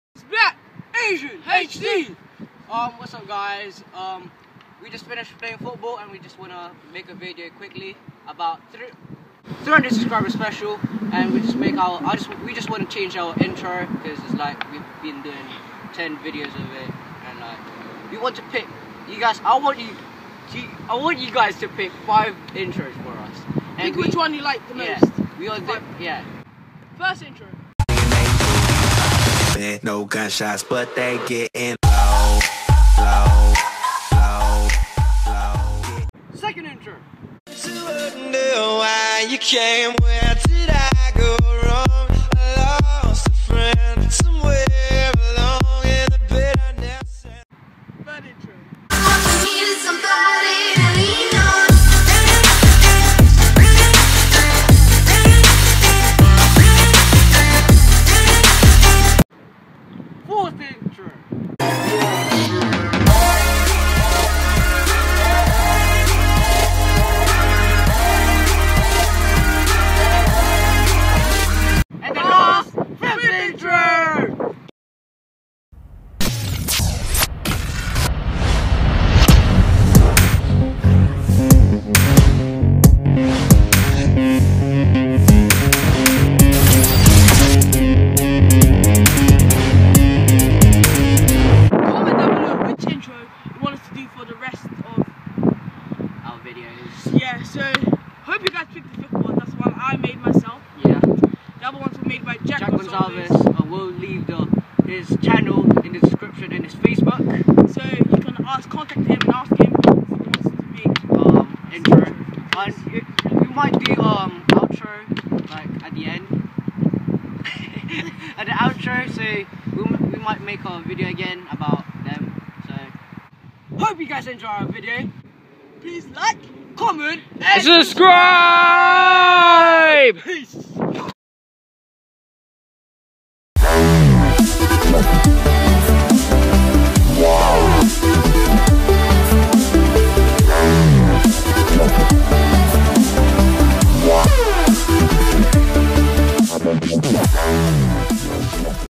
300 Subscribers Special!
Hey Guys! This is just a quick video as our Thanks for our 300 subscribers! We made a 5 Intros for you guys to pick!

Special thanks to Jack Gonsalves
If your want to contact him!